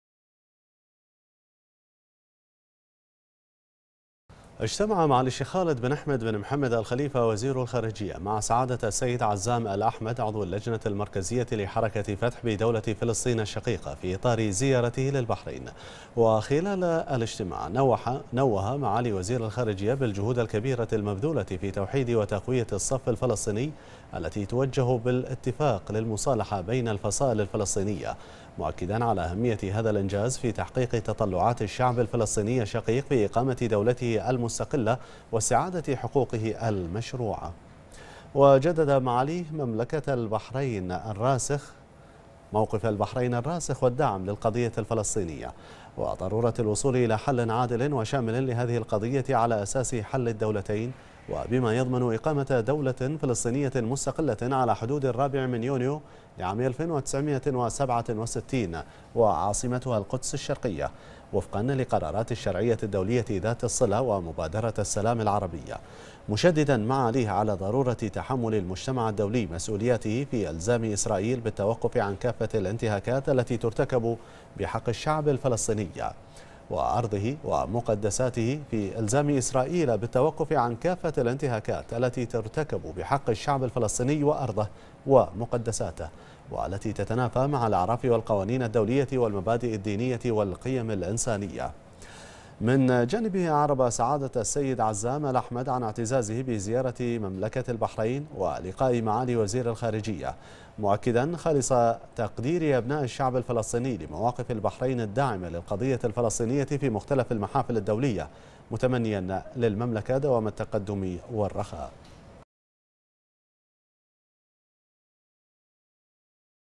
معالي وزير الخارجية يجتمع مع عضو اللجنة المركزية لحركة فتح بدولة فلسطين
المنامة في 16 نوفمبر / بنا / اجتمع معالي الشيخ خالد بن أحمد بن محمد آل خليفة، وزير الخارجية اليوم، مع سعادة السيد عزام الاحمد عضو اللجنة المركزية لحركة فتح بدولة فلسطين الشقيقة وذلك في إطار زيارته لمملكة البحرين.